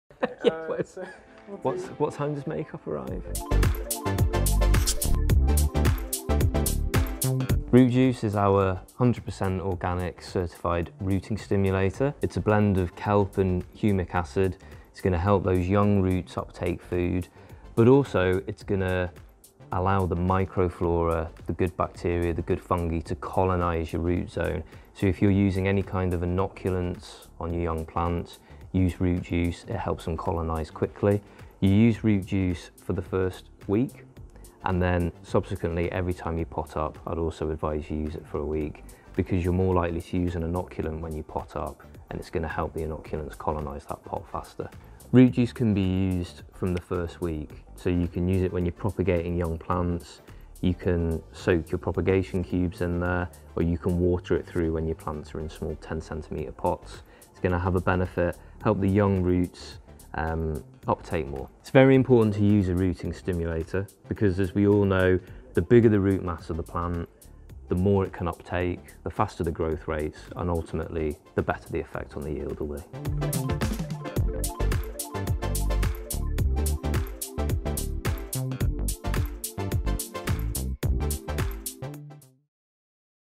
Biobizz Root Juice ft. Dan from Biobizz | DISCOVER
Promote root development and root health with 100% organic Root Juice. A small amount stimulates bacterial life in your rootzone, to keep roots healthy. Use this to prevent root diseases, root burning & overfeeding. You'll also get a denser root ball.